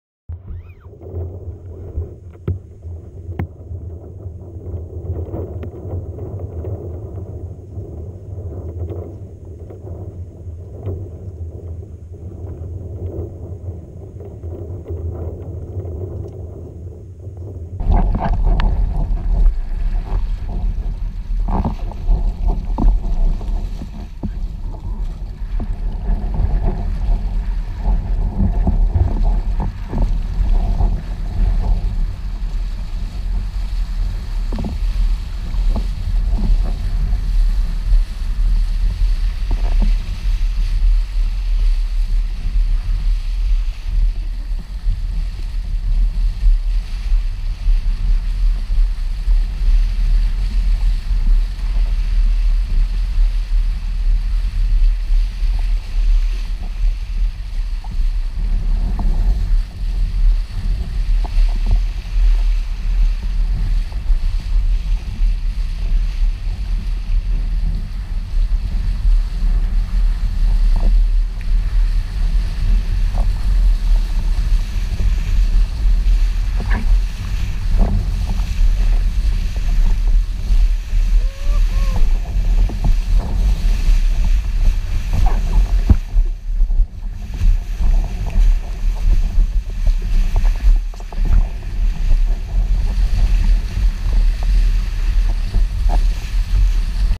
Windsurf - Lago Morto 2018
Windsurf Lago Morto
Leggera brezza mattutina con Vela Neilpryde Atlas 5.8 e Tavola JP Super Sport 113.
Vento 10-12 nodi da Nord